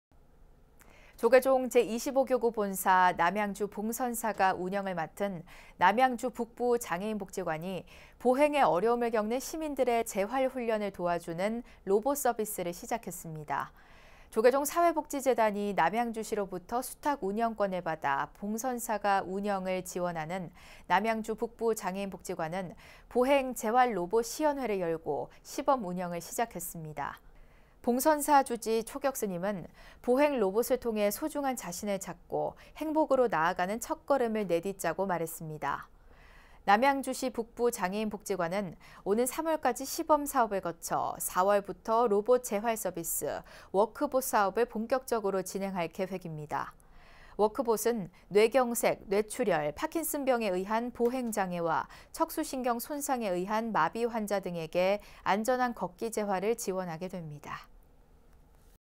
[BBS뉴스] 남양주 북부장애인복지관, ‘보행 로봇 재활 서비스’ 시작
조계종 제25교구본사 남양주 봉선사가 운영을 맡은 남양주북부 장애인복지관이 보행에 어려움을 겪는 시민들의 재활 훈련을 도와주는 로봇 서비스를 시작했습니다.

조계종 사회복지재단이 남양주시로부터 수탁운영권을 받아 봉선사가 운영을 지원하는 남양주 북부장애인복지관은 보행 재활로봇 시연회를 열고 시범 운영을 시작했습니다.

봉선사 주지 초격스님은 “보행 로봇을 통해 소중한 자신을 찾고 행복으로 나아가는 첫걸음을 내딛자”고 말했습니다.

남양주시북부장애인복지관은 오는 3월까지 시범 사업을 거쳐 4월부터 로봇재활서비스,워크봇 사업을 본격적으로 진행할 계획입니다.

‘워크봇’은 뇌경색, 뇌출혈, 파킨슨병에 의한 보행 장애와 척수신경 손상에 의한 마비 환자 등에게 안전한 걷기 재활을 지원하게 됩니다.